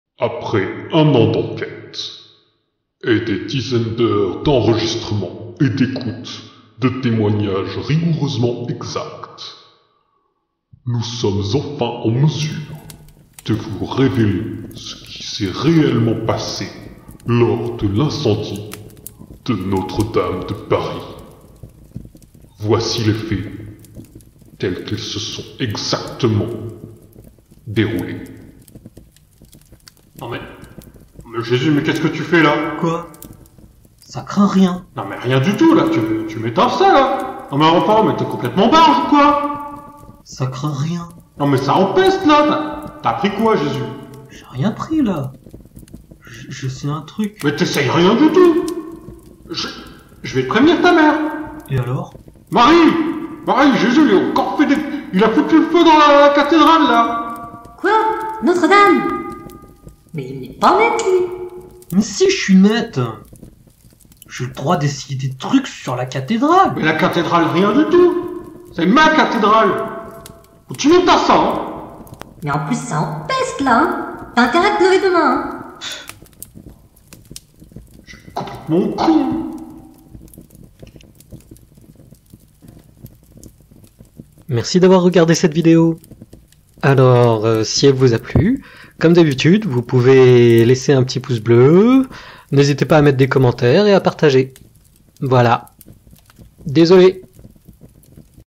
Jesus met le feu à sa chambre
Voici exactement 1 an que Notre Dame de Paris a été victime d'un odieux incendie.

Autant de temps d'une enquête poussée menée par des expert des plus grand complot terrestres  (extra et intra).

Voici la vérité dévoilée.

Outil de montage :
- KdeNLive (montage)
- Audacity (enregistrement et mixage)
- OpenToonz (Animation)
- Gimp (Couleur et détourage)
- Inkscape (Mise en page)